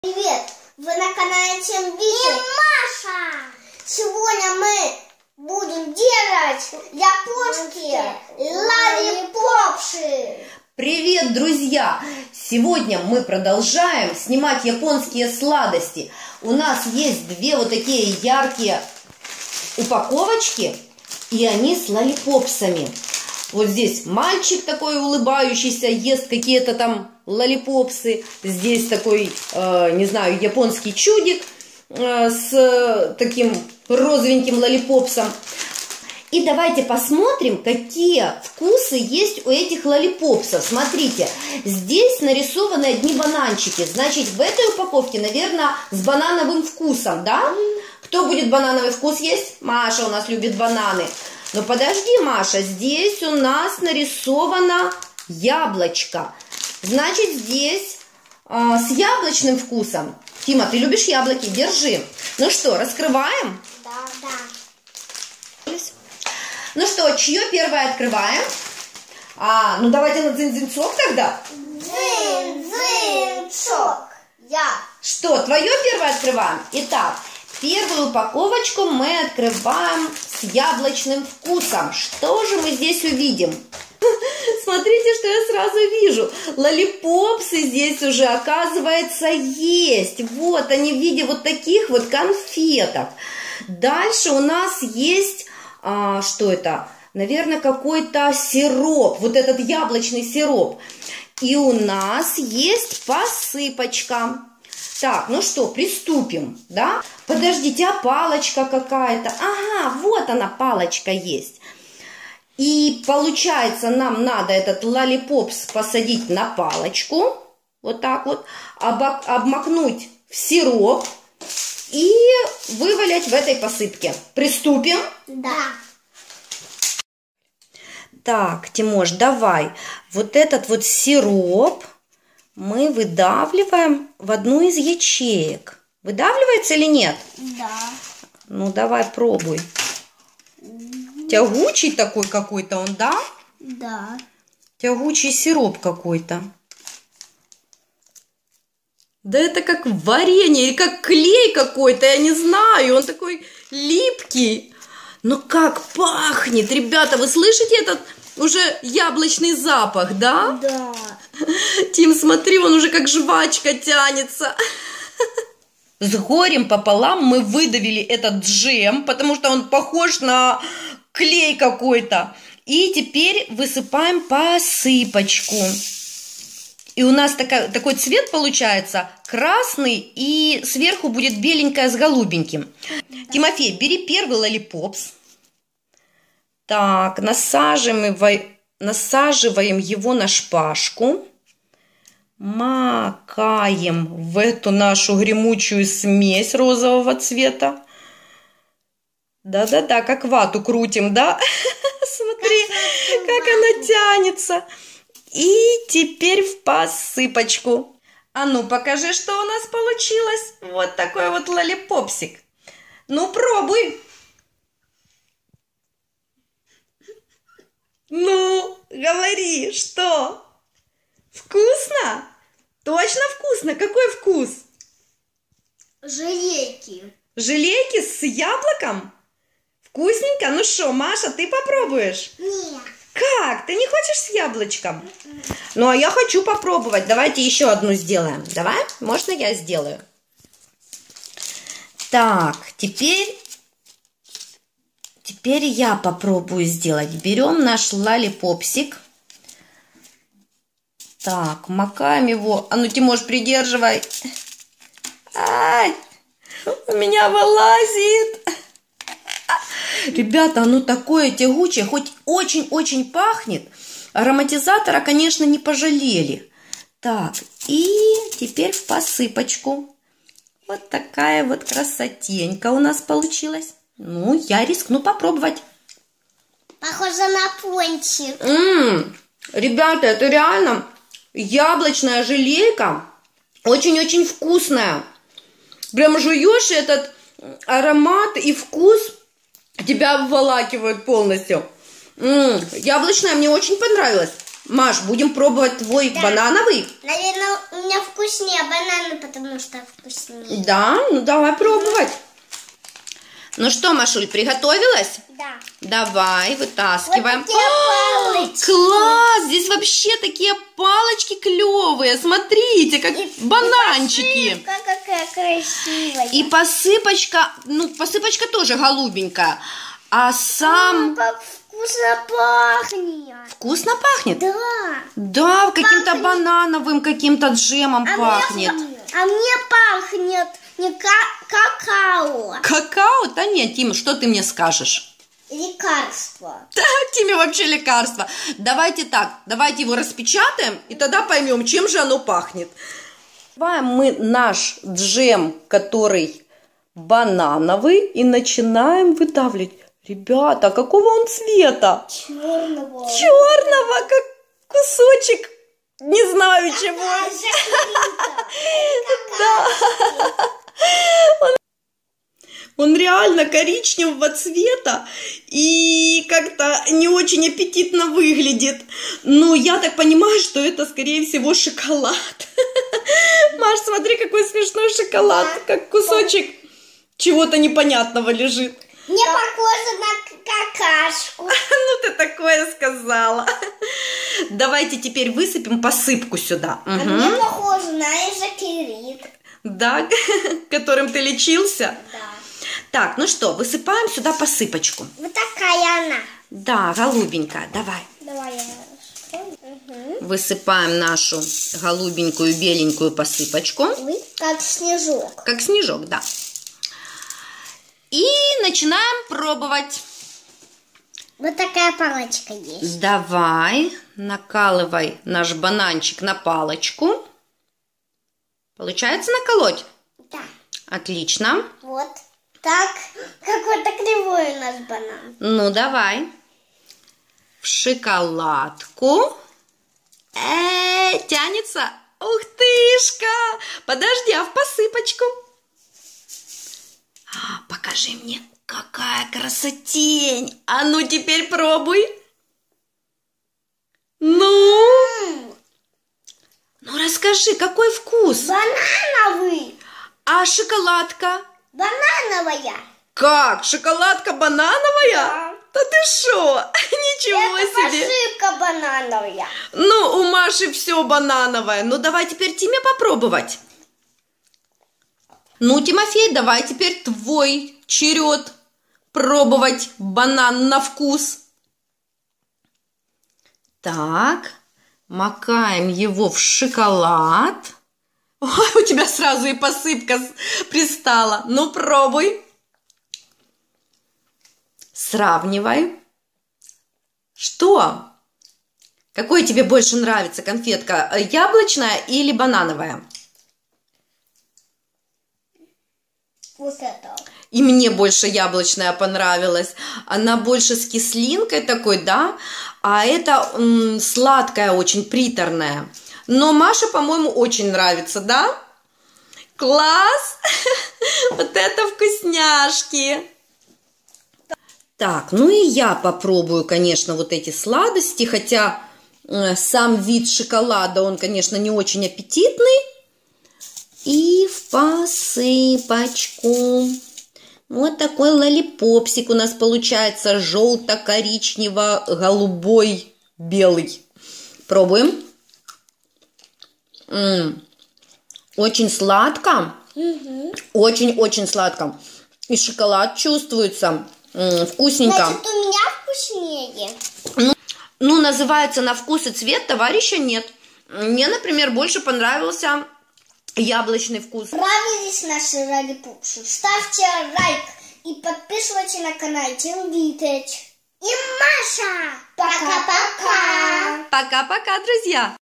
Японские сладости Вкусняшки Лолли попсы
Сегодня Тим Витыч и Маша распаковывают японские сладости - Лолли Попсы яблочные и банановые.  Японские вкусняшки всегда непредсказуемые - яблочный джем как клей, а шоколад как как какашка)))